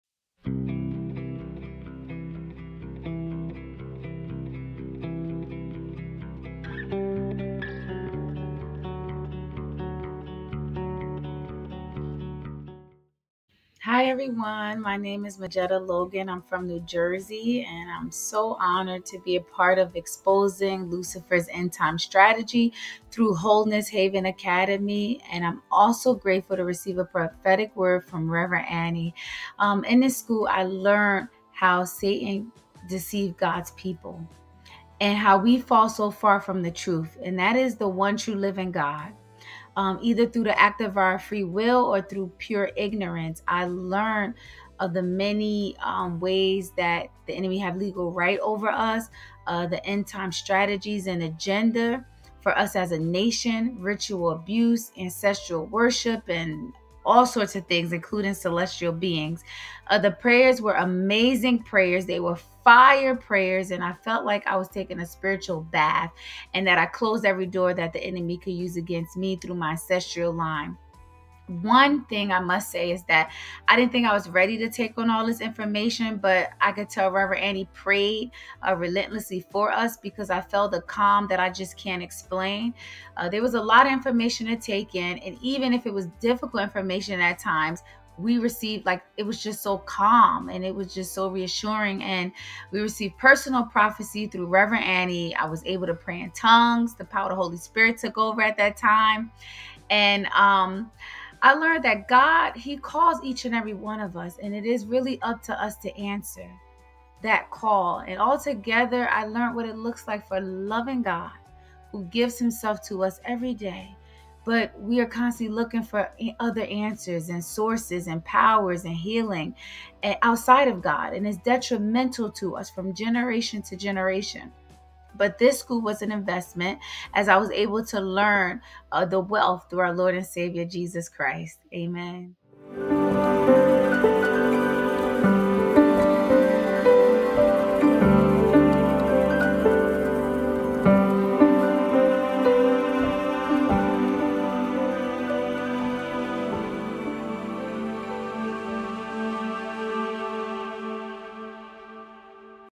Majetta's Breakthrough Testimony: Exposing Lucifer's End Time Strategies
We had the privilege of awarding graduation diplomas to students in 5 countries who completed our Exposing Lucifer's End Time Strategies certification school. Finishing our school was such a HUGE accomplishment, as each graduate completed a 5 month intense boot camp of 7 courses, 46 lessons with deliverance prayers, exercises and assignments.

Each graduate from our school has powerful testimonies of breakthroughs they want to share with you. We encourage you to watch this testimony from our graduate Majetta to have your faith increased and celebrate what God did!

We have videos available for purchase and download, which you can watch on your own time to learn about the topics.